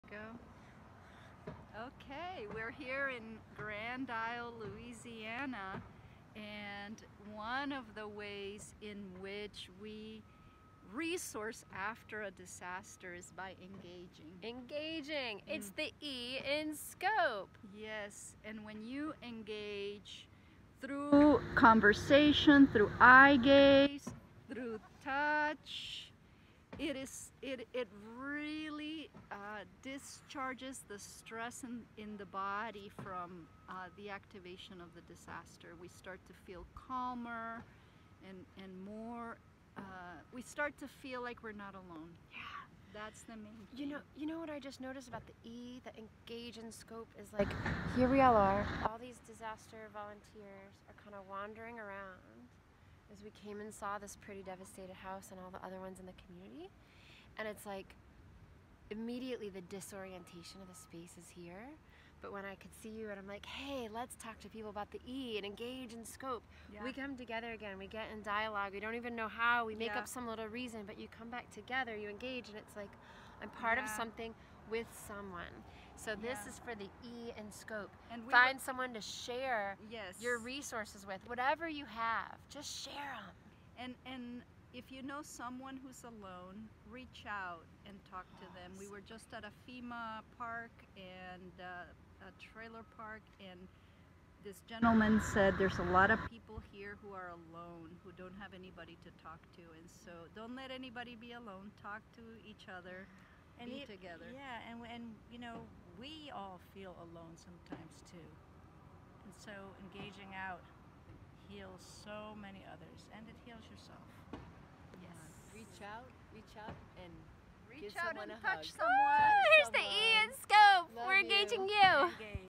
Engagement for Self Regulation
Crisis stabilization and safety skills after a disaster.